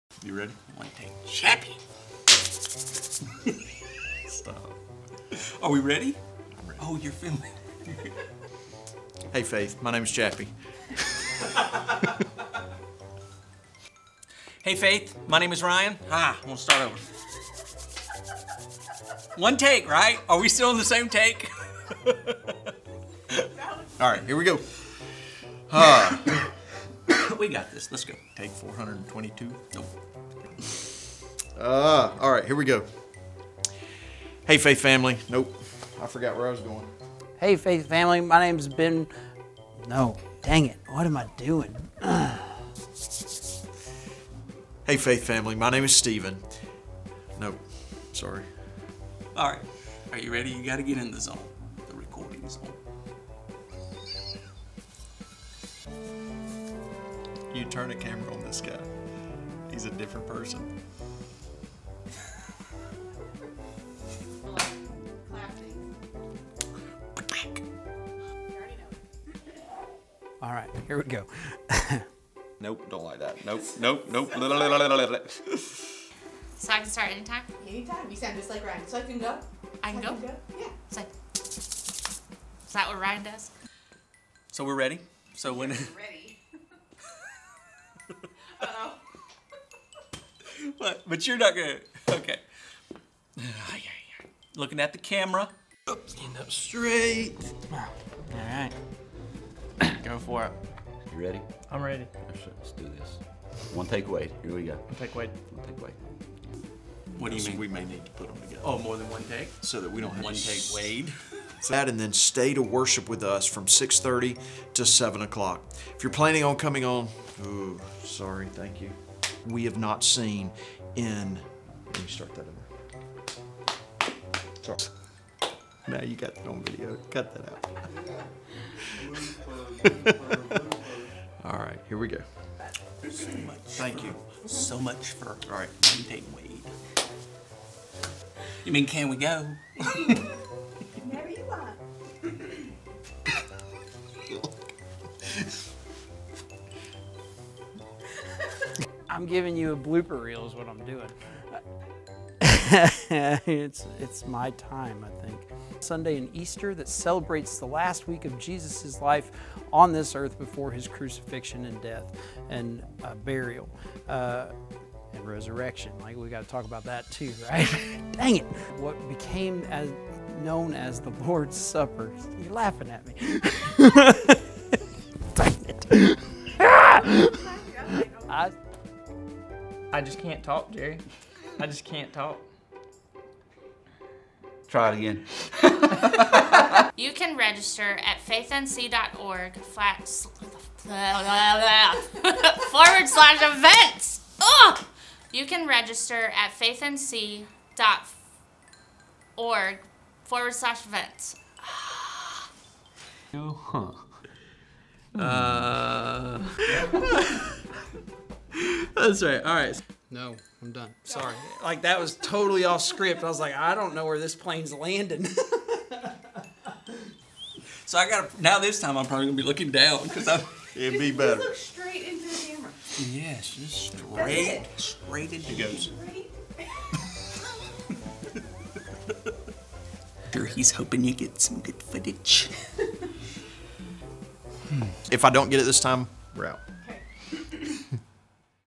Blooper Reel
Our pastors and staff always work hard to bring you great video resources and important information, but we don't always get it quite right on the first take (and we certainly don't take ourselves too seriously).

Please enjoy this compilation of bloopers we've collected over the last few months.